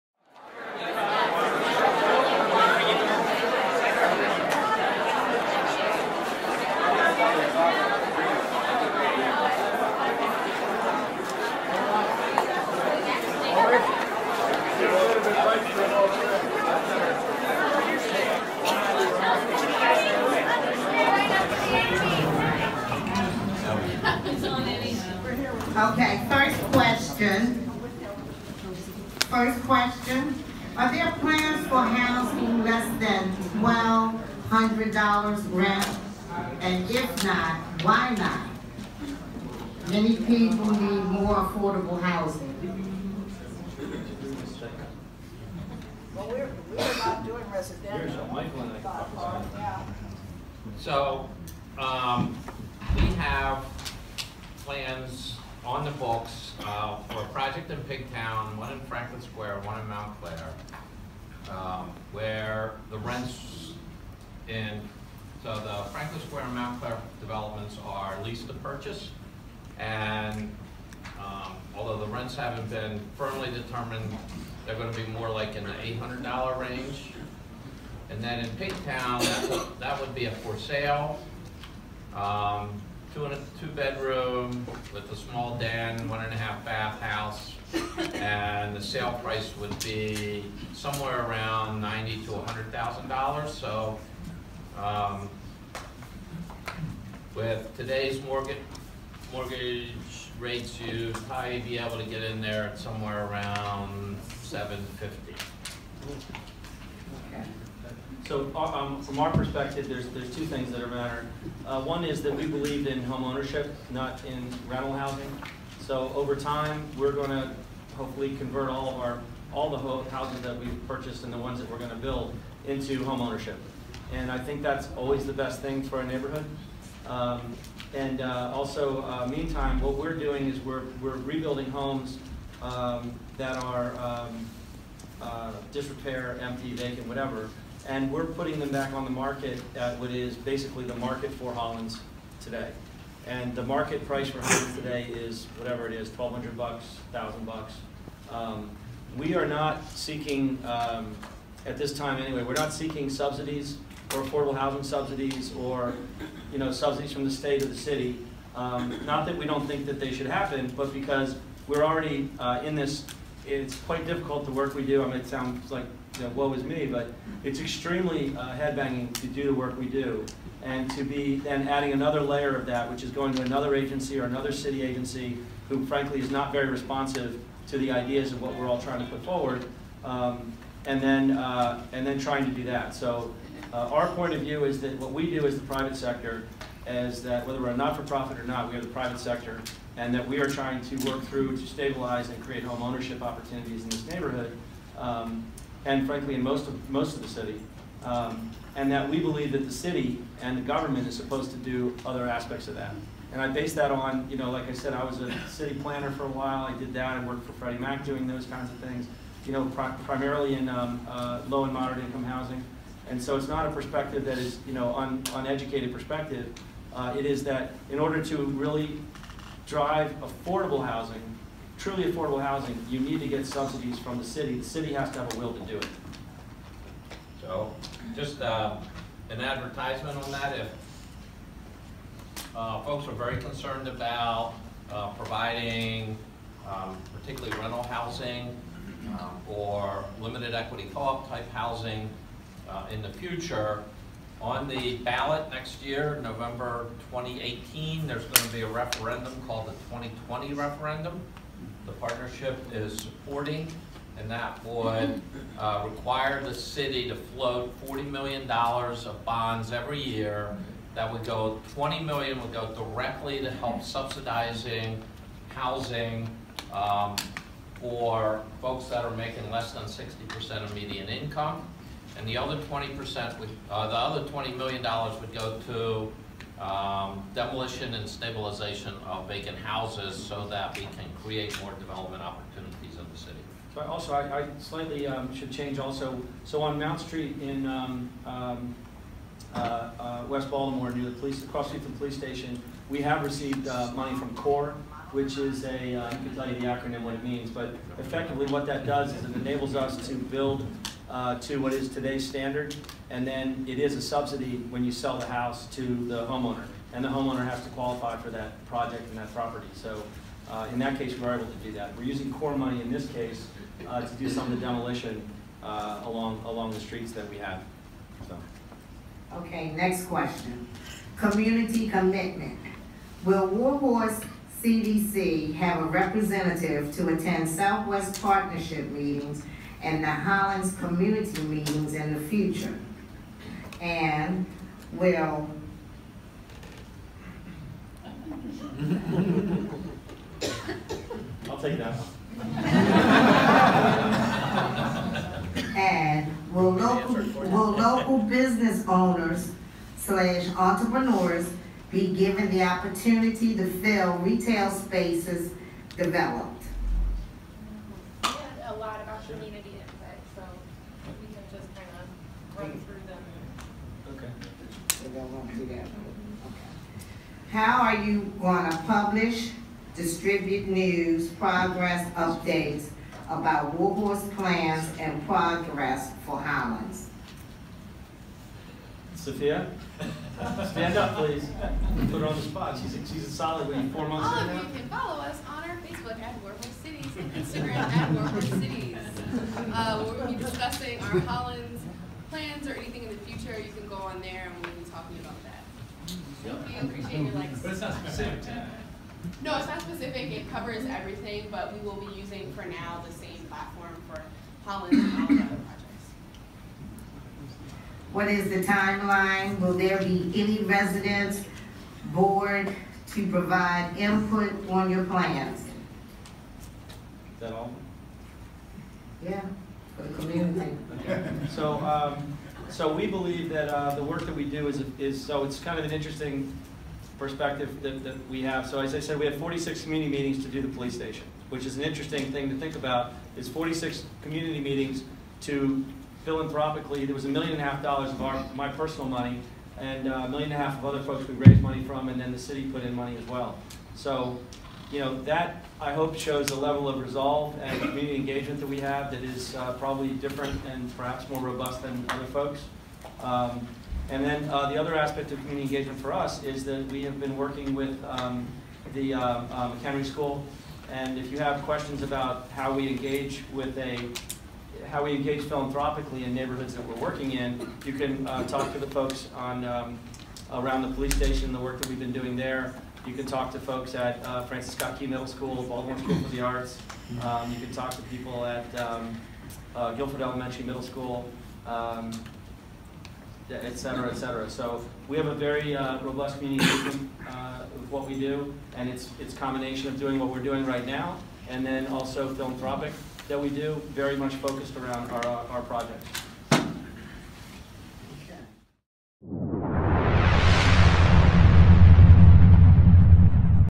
J Scott Plank Addresses Baltimore Residents Concerns PT 1
11/8/2017 J.Scott Plank, CEO of War Horse, answers questions from Baltimore's Hollins Market, Franklin Square, Union Square and adjoining communities affected by the $6million neighborhood re-development project.